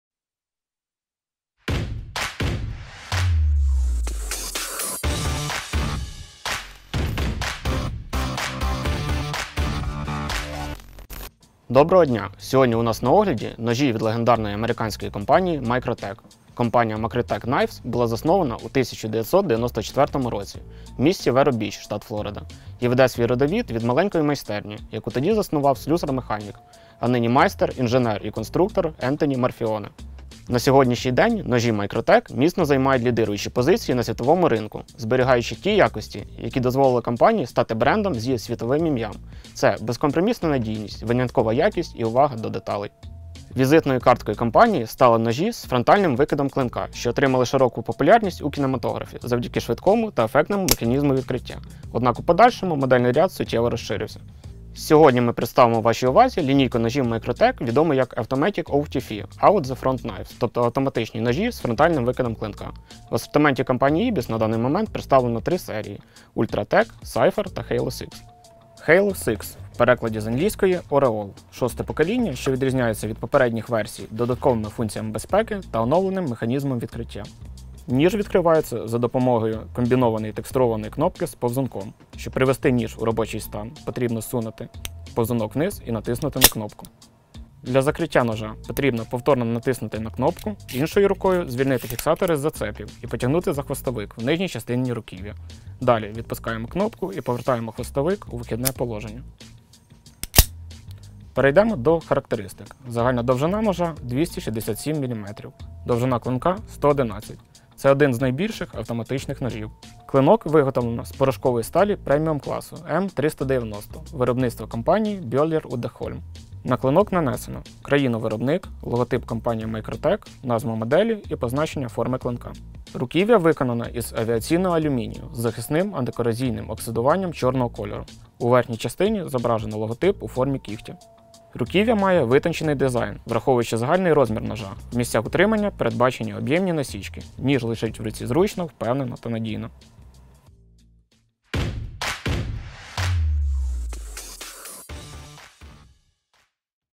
Складаний ніж Microtech Halo VI
В даному відео огляді ми розглядаємо унікальні ножі від компанії Mictotech. Неординарна новинка 2018 року - складані ножі з фронтальним викидом клинка Microtech Halo VI. Вони відрізняються від інших моделей додатковою функцією безпеки для запобігання випадкового викиду клинка, виготовленого з порошкової нержавіючої сталі М390.